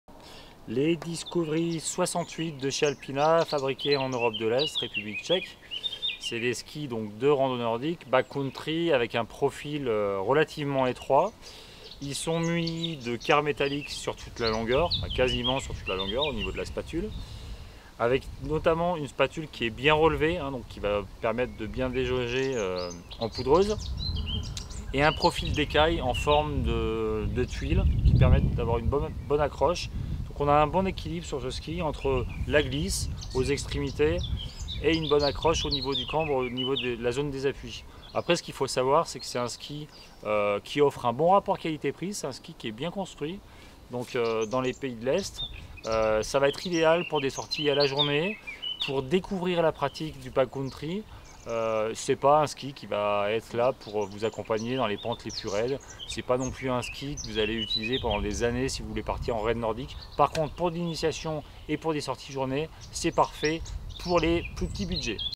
Alpina Discovery 68 - Ski de Randonnée Nordique
Présentation des Skis de Randonnée Nordique Alpina Discovery 68. Retrouvez les compléments d'informations de ces skis Backcountry sur notre fiche technique